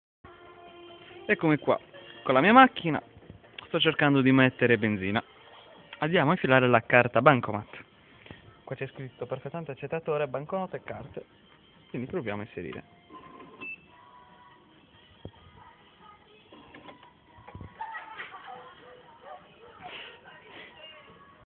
Rifornimento Impossibile
Distributore di Carburante Q8 di Marritza, sulla strada da Porto Torres a Castelsardo, vado ad inserire la mia carta bancomat ma succede una cosa che ha dell'incredibile...

Petrol Q8 of Marritza, near Porto Torres in Sardinia. I'm going to insert my ATM Card but it happens something incredible...

Fun Risate Divertente